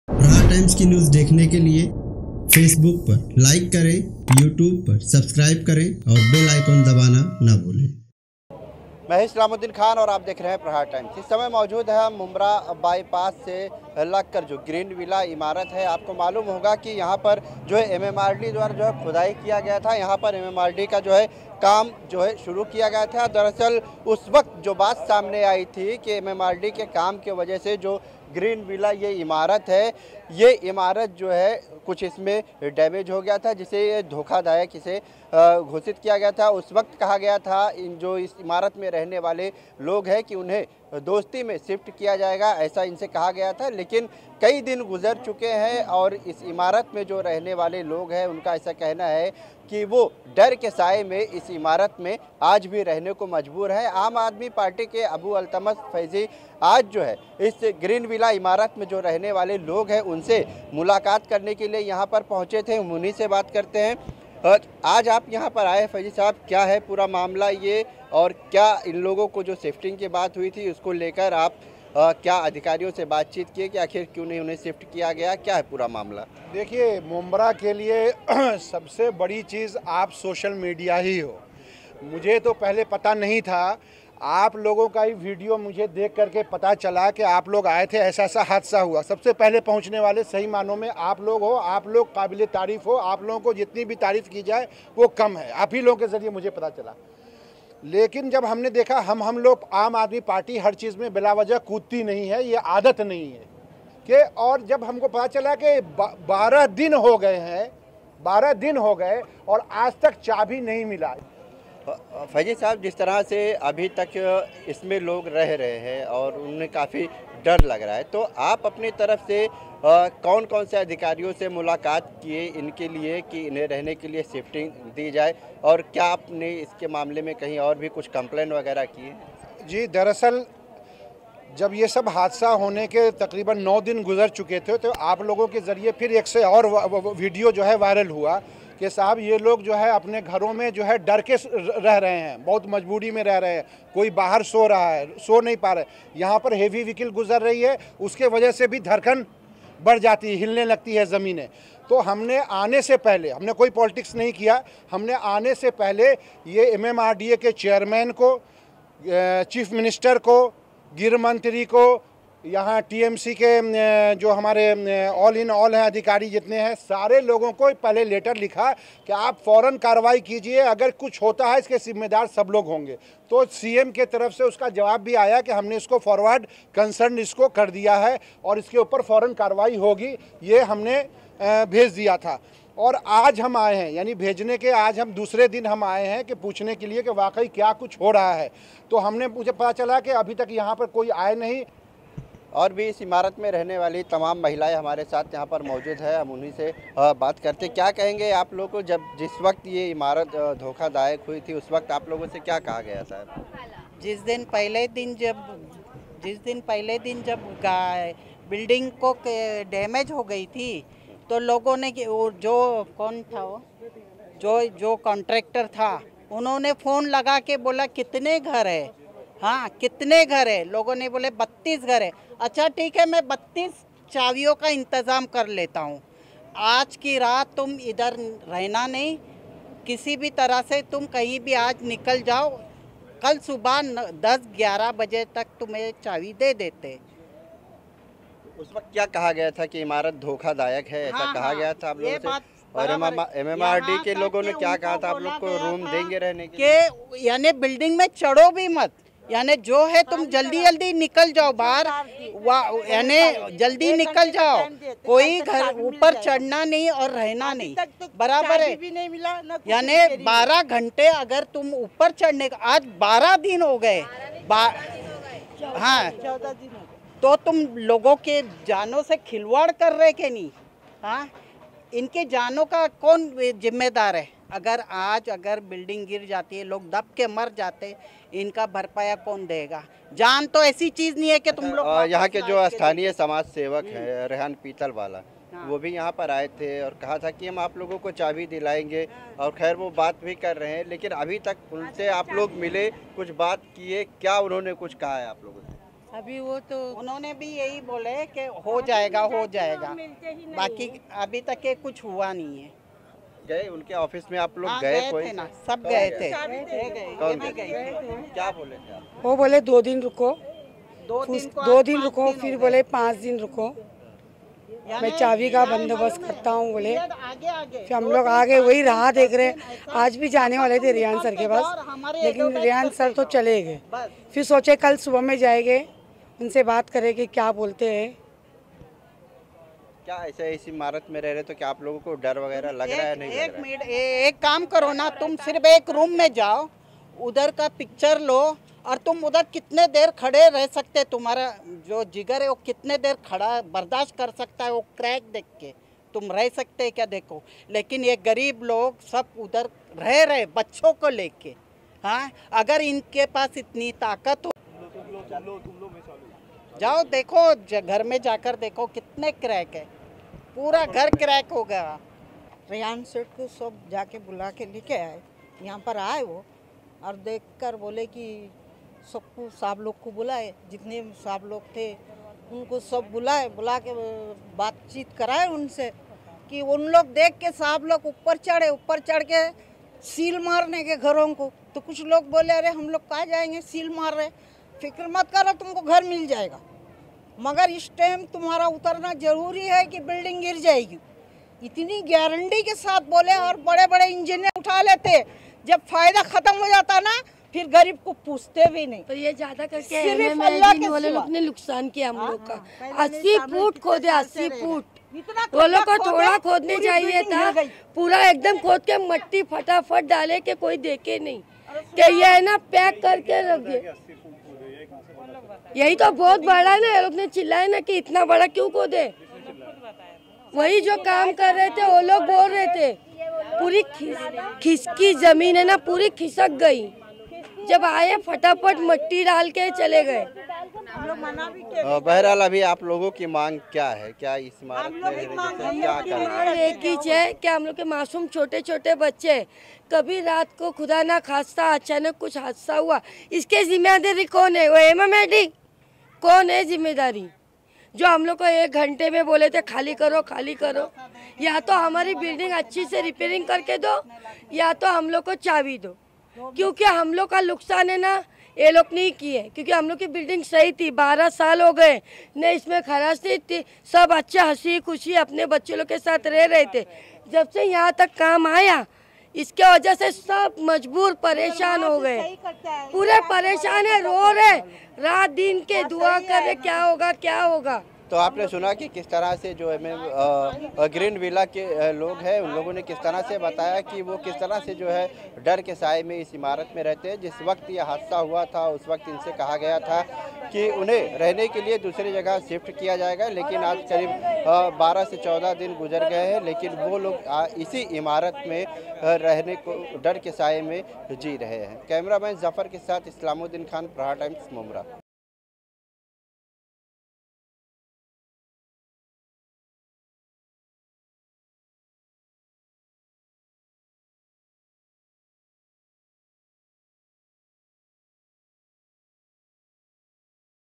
डर के साये में ग्रीन वीला के लोग AAP के फैजी ने दिया लेटर || Islamuddin Khan || Aapka Prahar Times
डर के साये में ग्रीन वीला के लोग AAP के फैजी ने दिया लेटर || Islamuddin Khan

MMRDA के काम के दौरान ग्रीन  वीला नामक इमारत धोखा दायक हो गई थी इस इमारत के लोग शिफ्टिंग की मांग कर रहे है